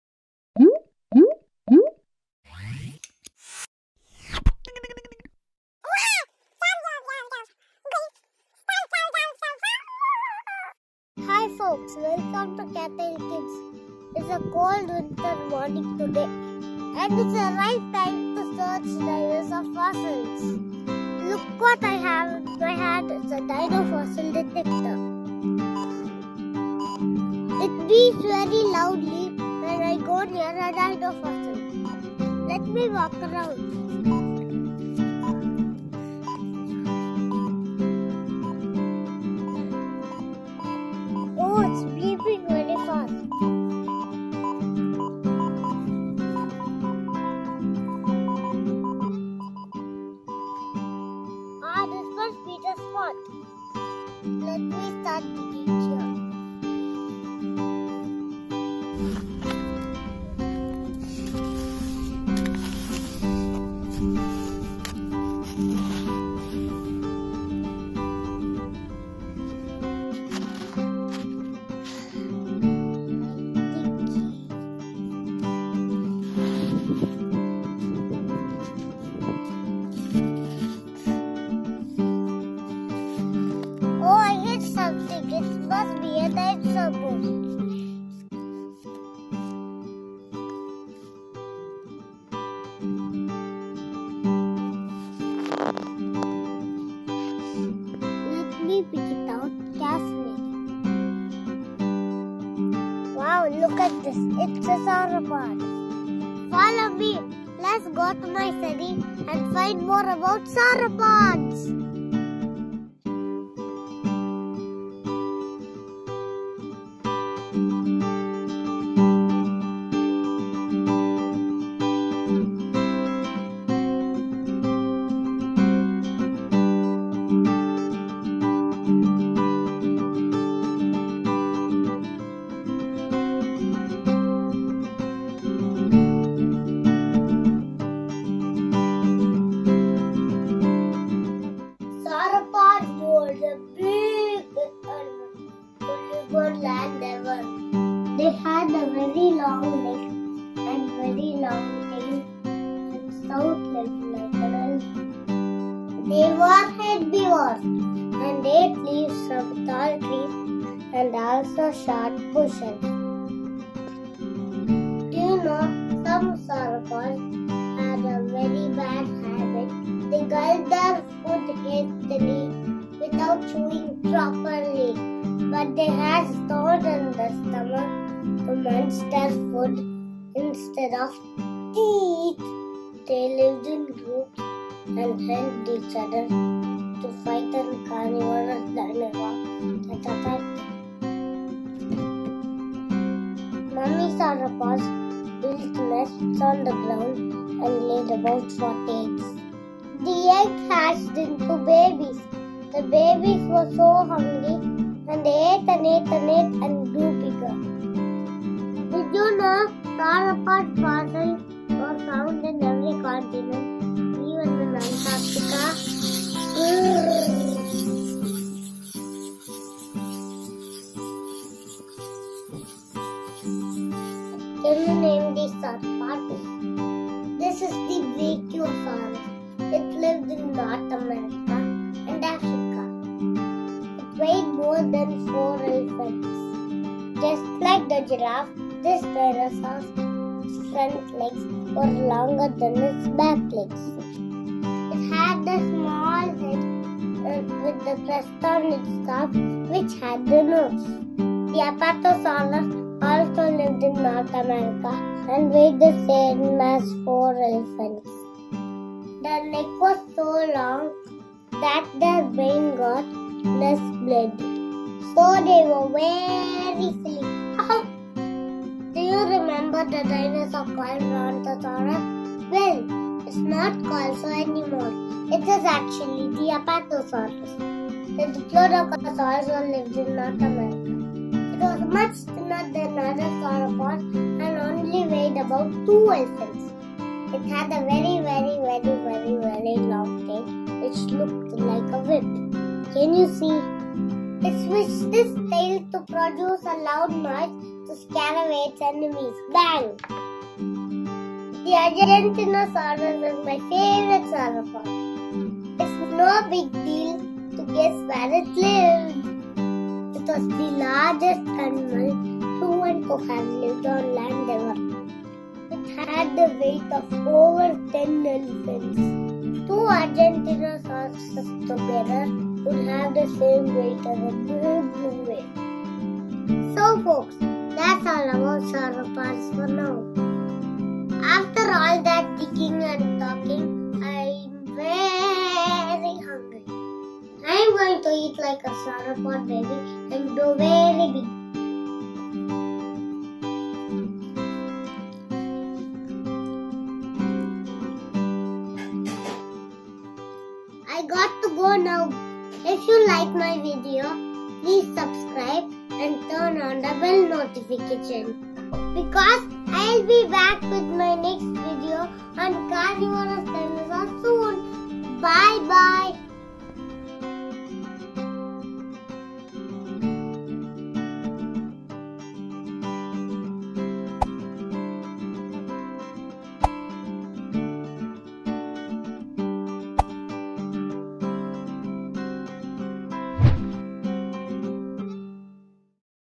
Biggest dinosaur in the world | Dinosaur fossil hunt
Watch Rithul excavate dinosaur fossils and speak about the giant herbivorous dinosaurs. Did you know the Argentinosaurus was the biggest dinosaur to live on land?